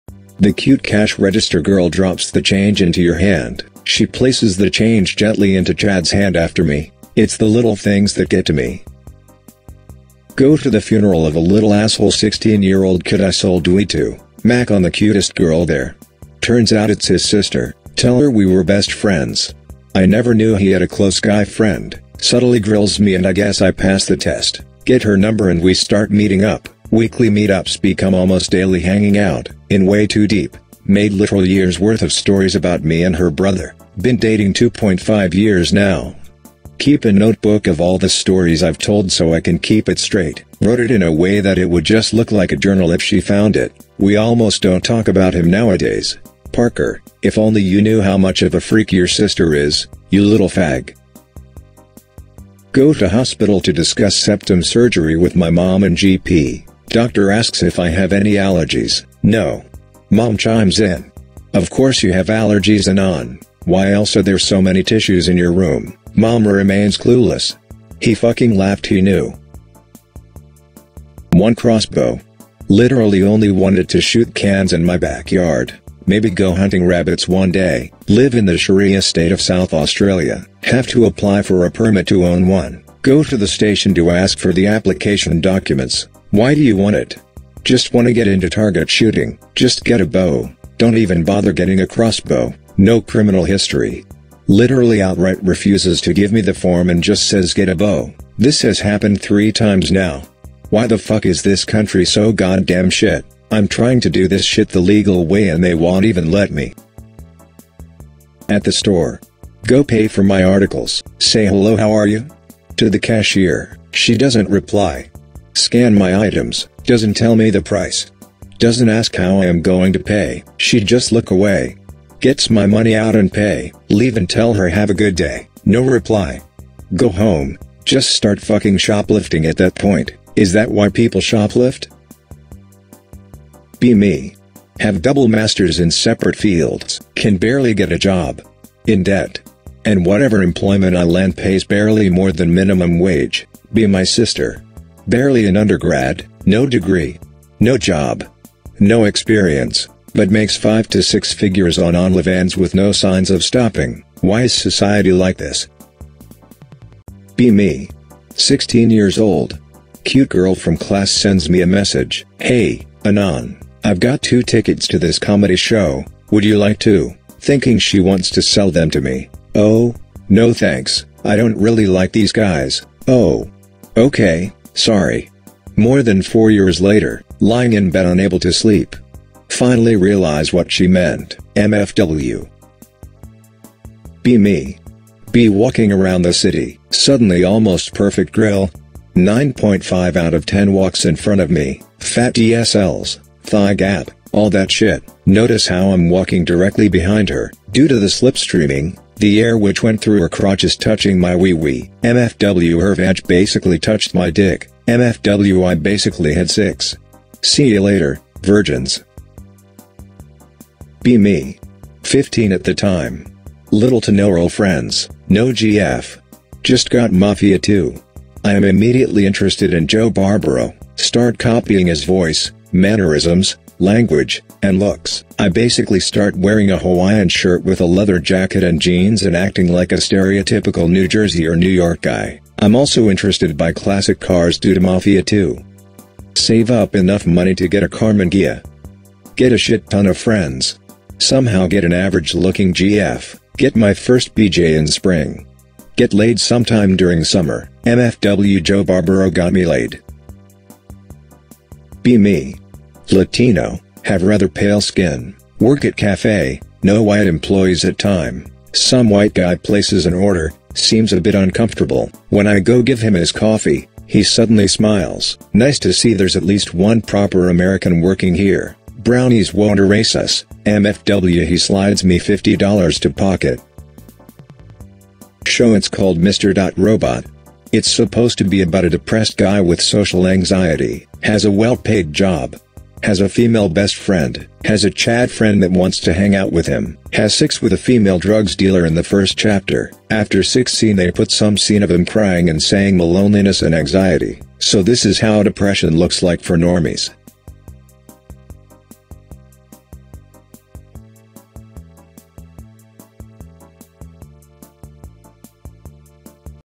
ANON REALIZES HE ISN'T A CHAD - BEST OF R/GREENTEXT #70
Yet another collection of the latest and funniest greentexts.

I try to upload daily.

Like and Sub would be greatly appreciated :)